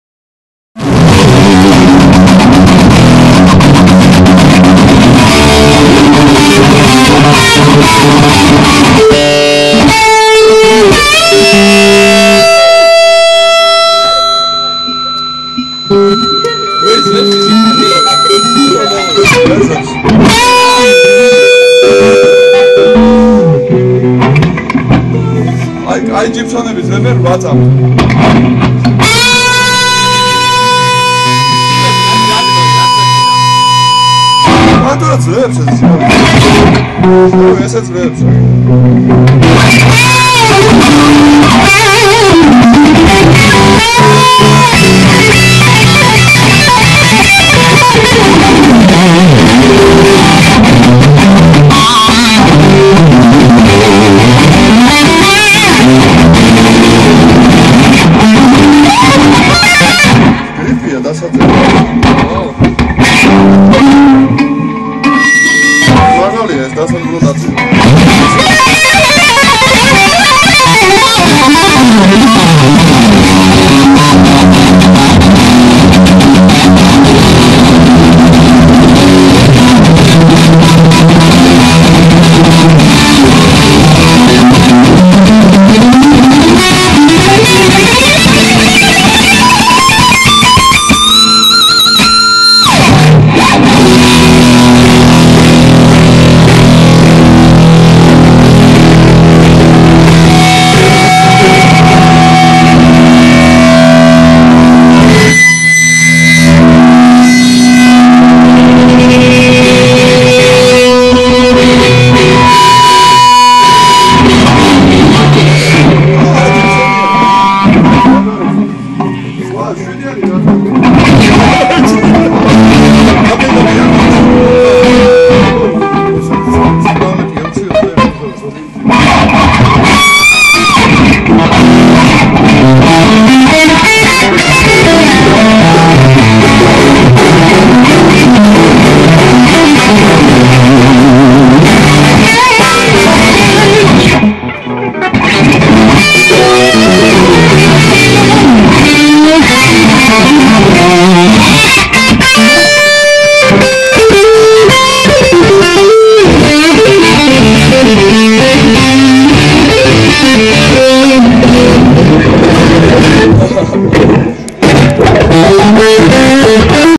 Gia Peikrishvili & Akaki Surguladze GIBSON test
High-class musician Checking guitar .GIBSON LES PAUL STUDIO FADED
Sorry for bad voice